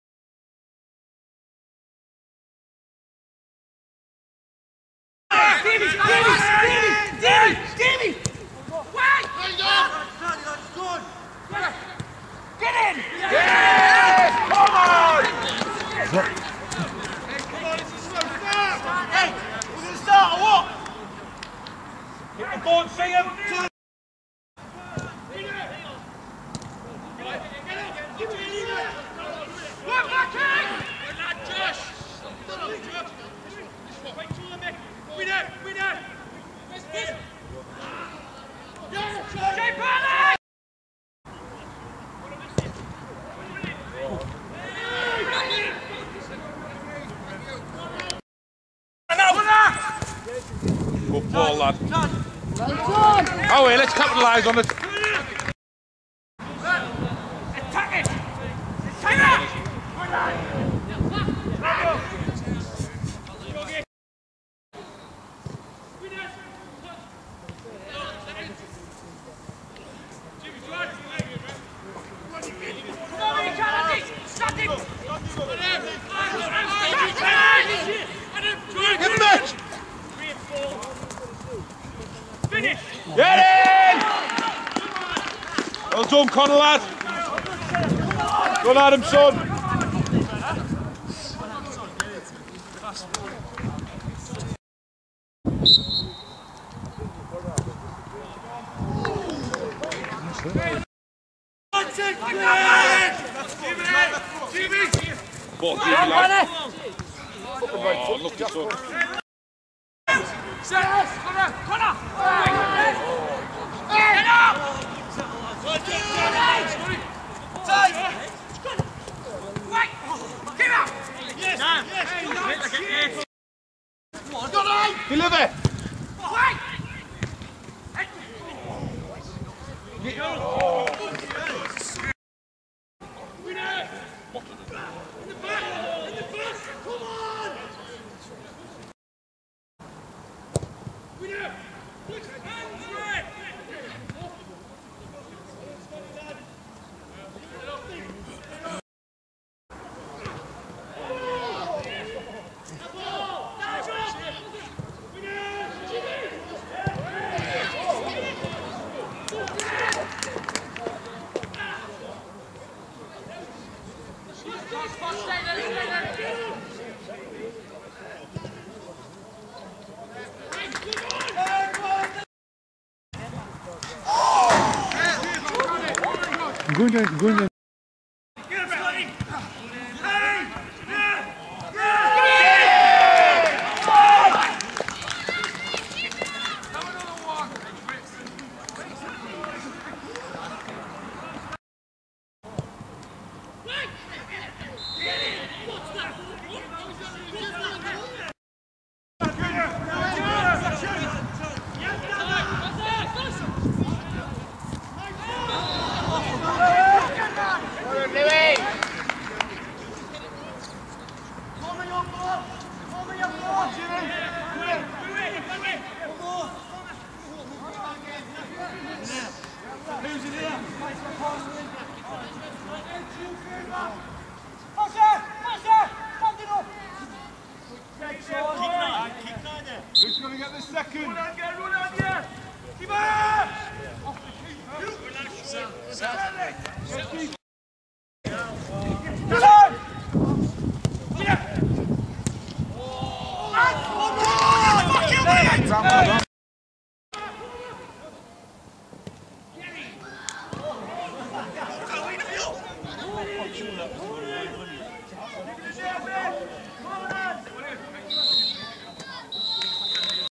Darlington RA v Seaham Red Star
Durham Challenge Cup 1st Round
29th October 2013

FT  Darlington RA 1-2 Seaham Red Star
      Ramsbottom Aa    Winter (2)